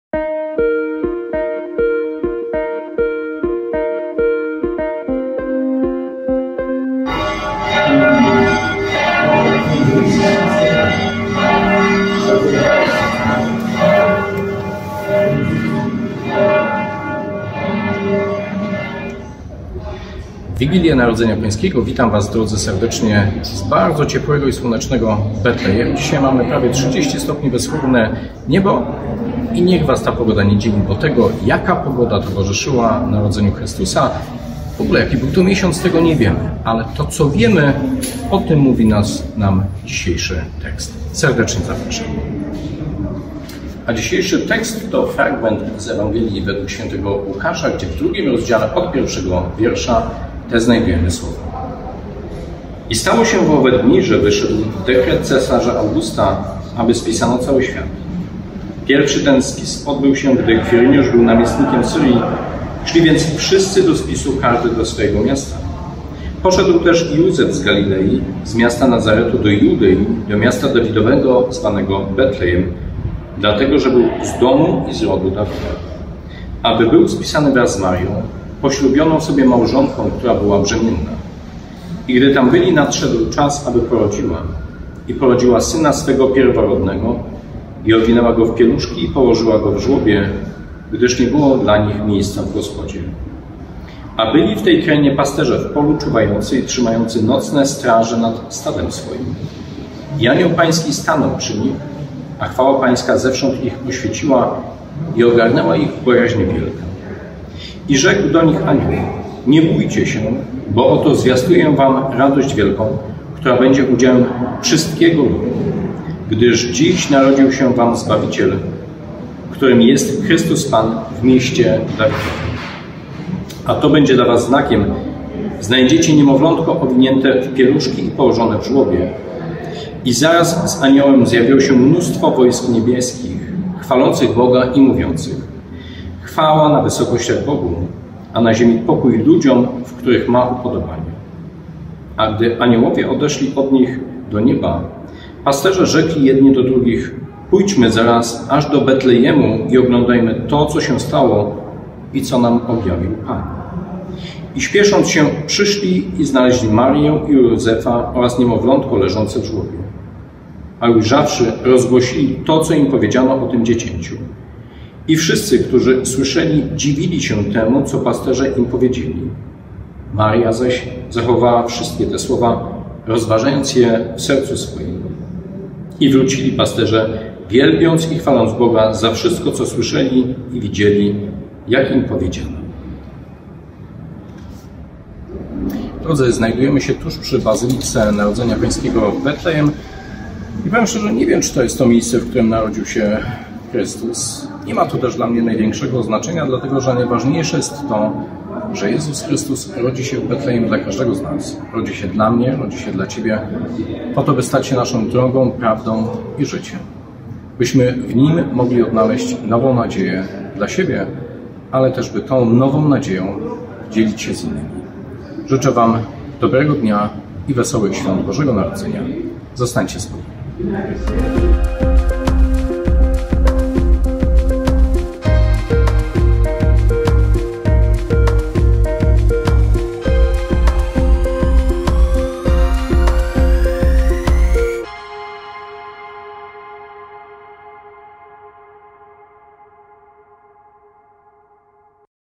Codziennik biblijny, Słowo na dzień 24 grudnia 2022 r.
Rozważanie biblijne na Słowo dnia:

I stało się w owe dni, że wyszedł dekret cesarza Augusta, aby spisano cały świat.
Pierwszy ten spis odbył się, gdy Kwiryniusz był namiestnikiem Syrii. Szli więc wszyscy
do spisu, każdy do swego miasta. Poszedł też i Józef z Galilei, z miasta Nazaretu,
do Judei, do miasta Dawidowego, zwanego Betlejem, dlatego że był z domu i z rodu Dawida,
aby był spisany wraz z Marią, poślubioną sobie małżonką, która była brzemienna. I gdy
tam byli, nadszedł czas, aby porodziła. I porodziła syna swego pierworodnego, i owinęła
go w pieluszki, i położyła go w żłobie, gdyż nie było dla nich miejsca w gospodzie.
A byli w tej krainie pasterze w polu czuwający i trzymający nocne straże nad stadem
swoim. I anioł Pański stanął przy nich, a chwała Pańska zewsząd ich oświeciła;
i ogarnęła ich bojaźń wielka. I rzekł do nich anioł: Nie bójcie się, bo oto zwiastuję
wam radość wielką, która będzie udziałem wszystkiego ludu, gdyż dziś narodził sięwam
Zbawiciel, którym jest Chrystus Pan, w mieście Dawidowym. A to będzie dla was znakiem:
Znajdziecie niemowlątko owinięte w pieluszki i położone w żłobie. I zaraz z aniołem
zjawiło się mnóstwo wojsk niebieskich, chwalących Boga i mówiących: chwała na
wysokościach Bogu, a na ziemi pokój ludziom, w których ma upodobanie. A gdy aniołowie
odeszli od nich do nieba, pasterze rzekli jedni do drugich: Pójdźmy zaraz aż do
Betlejemu i oglądajmy to, co się stało i co nam objawił Pan. I śpiesząc się, przyszli,
i znaleźli Marię i Józefa oraz niemowlątko leżące w żłobie. A ujrzawszy, rozgłosili to,
co im powiedziano o tym dziecięciu. I wszyscy, którzy słyszeli, dziwili się temu, co
pasterze im powiedzieli. Maria zaś zachowywała wszystkie te słowa, rozważając je w sercu
swoim. I wrócili pasterze, wielbiąc i chwaląc Boga za wszystko, co słyszeli i widzieli,
jak im powiedziano.
Ewangelia Łukasza 2,1-20
Codziennie o 7 rano spotkanie z Biblią.